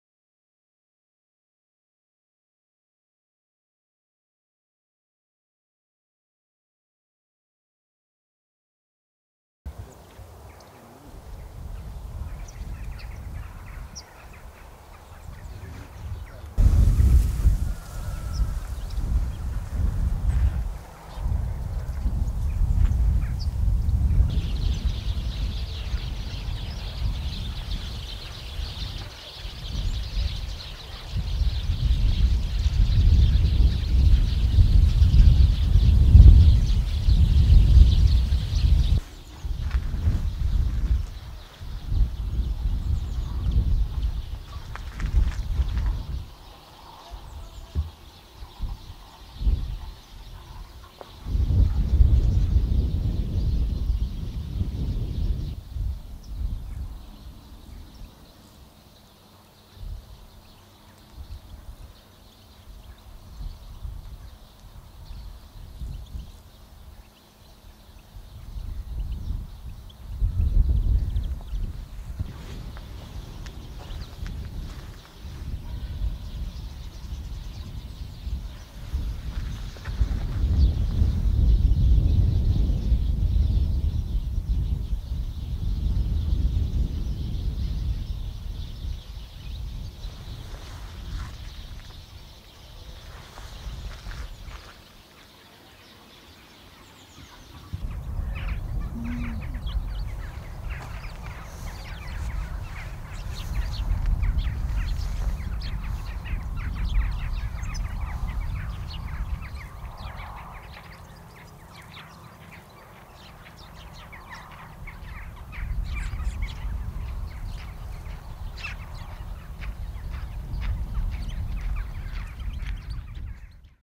STEGLITS European Goldfinch (Carduelis carduelis) Klipp - 2623
Steglits. Art-Video. European Goldfinch in Sweden,  AROS FILM Fotograf: Gunnar Fernqvist. Västerås. Sweden.  (L-28.)